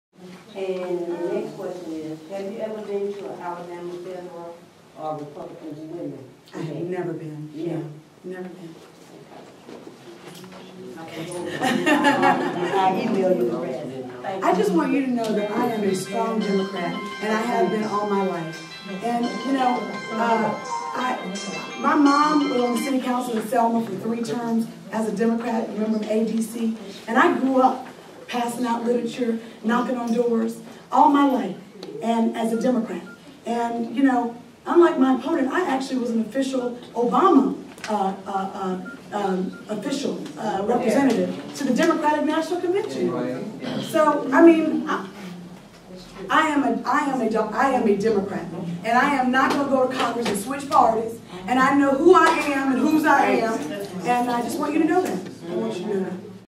Sewell Town Hall - "I'm a Democrat"
Terri Sewell, Democratic candidate for Congress in Alabama's 7th congressional district, fields a question as to whether she's been to a meeting of the Alabama Federation of Republican Women.  The answer is NO.